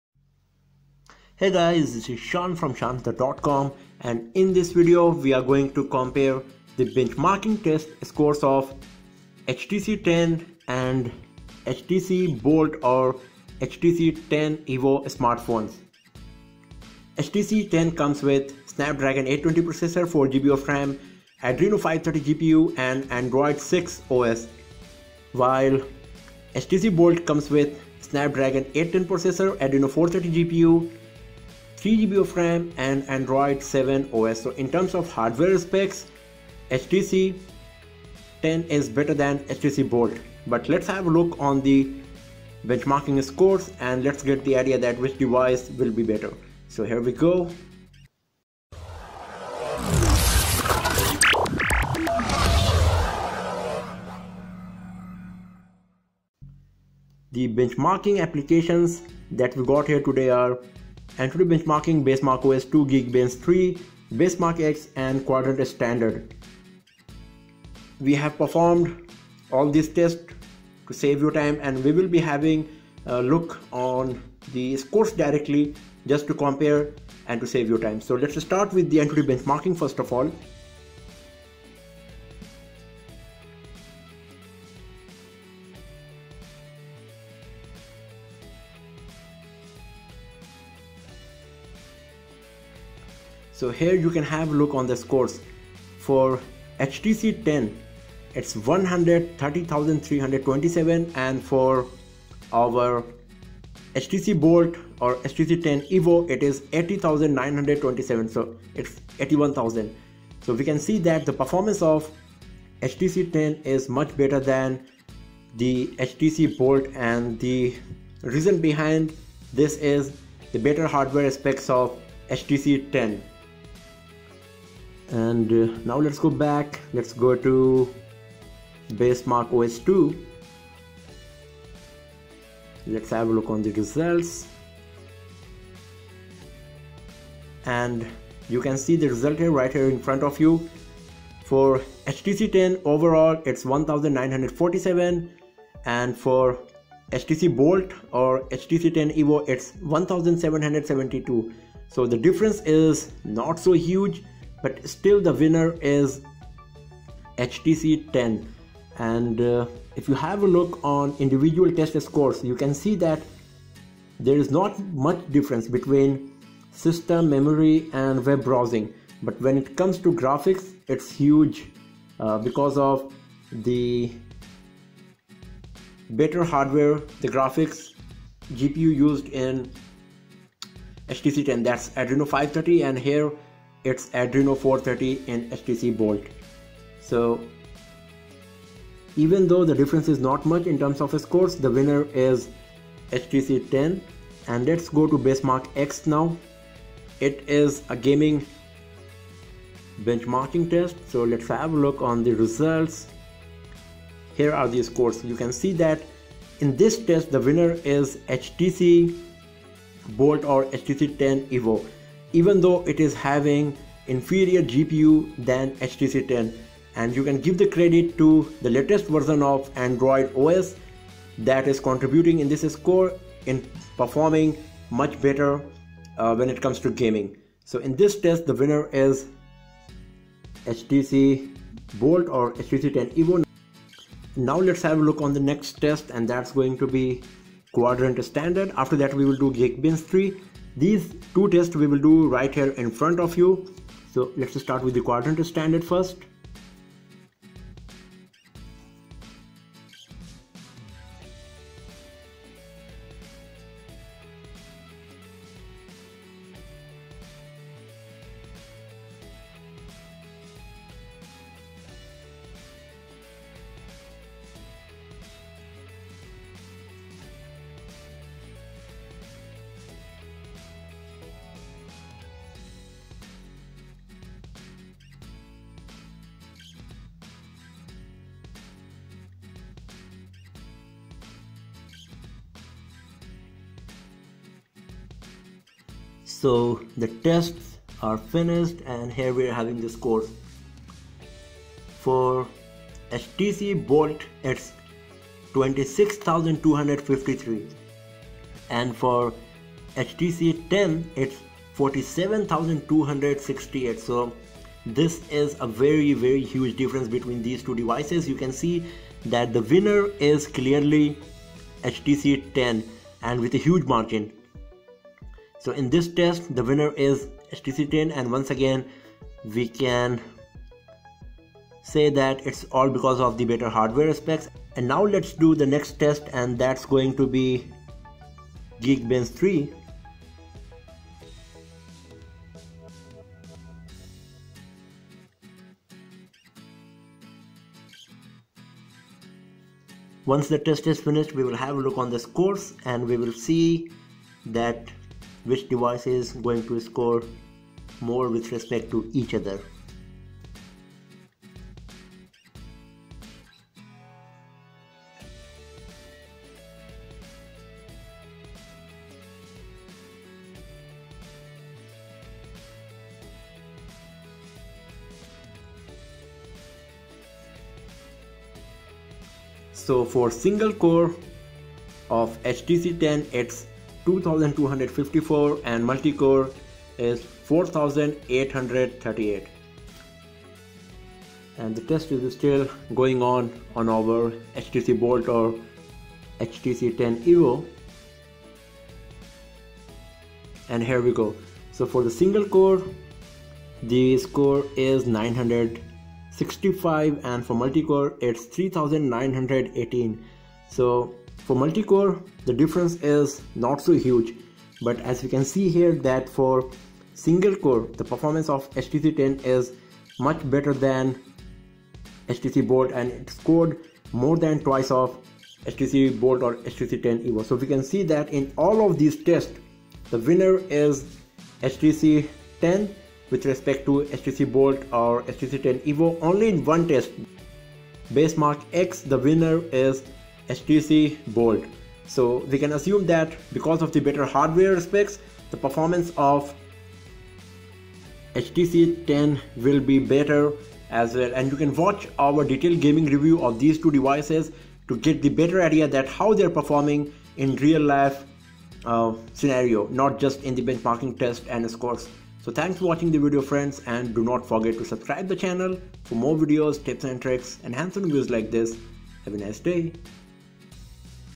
HTC 10 vs HTC 10 evo / HTC Bolt - BENCHMARK COMPARISON
Detailed benchmark tests comparison between HTC 10 and HTC 10 evo / HTC Bolt smartphones.